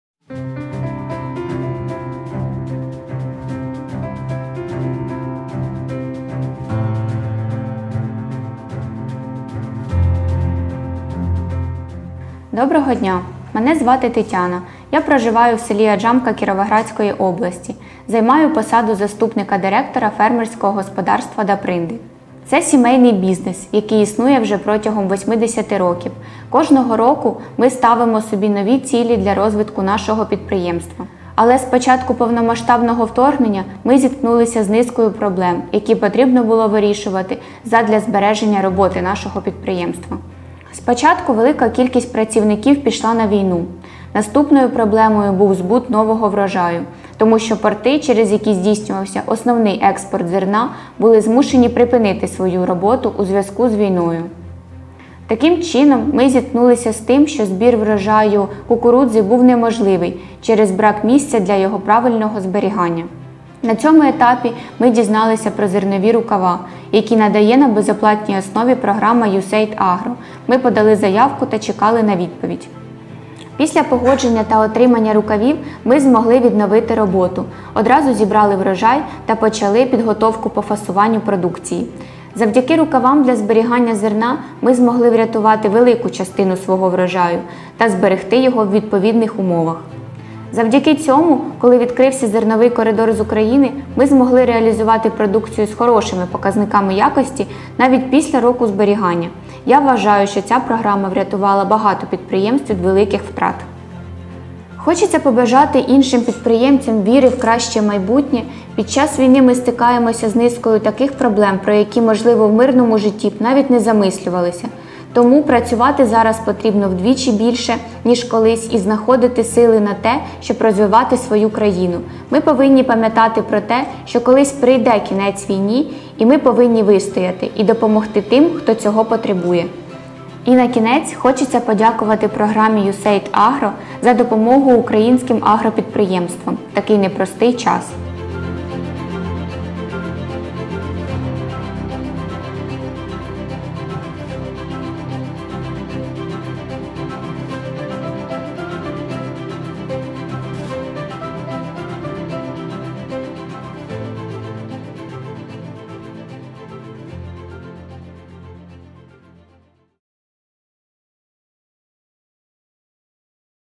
ФГ Дапринди С. М.: зберегли врожай в зернових рукавах від Програми USAID АГРО
Багаторічну роботу та розвиток сімейного фермерського господарства з Кіровоградщини поставила під загрозу війна.

Спершу, кваліфіковані працівники ФГ Дапринди С. М. стали на захист країни. Згодом, вирощений у надскладних умовах врожай не змогли реалізувати через зупинку морських портів. А збір кукурудзи став неможливим через нестачу місця для зберігання.

У пошуку рішень господарство дізналося про можливість зберігання в полімерних рукавах, які у 2022 році Програма USAID АГРО передала для допомогти невеликим агровиробникам. Так, в межах Ініціативи USAID на підтримку стійкості агросектору України, AGRI-Україна, АГРО передала понад 8,000 рукавів для зберігання зерна, 29 зернопакувальних машин та бункерів-перенавантажувачів, а також підтримала встановлення модульних сховищ/ангарів для зберігання зерна загальною місткістю 77 тис тон. Всього це допоможе зберегти 1,7 мільйонів тонн зерна.

Поступово, агровиробники вже розпаковують збережене зерно, продають його на внутрішній ринок та на експорт. З одного боку, підтримують стале надходження сільгосппродукції до покупців, а з іншого - забезпечують роботу своїх господарств необхідними ресурсами для польових робіт в новому сезоні.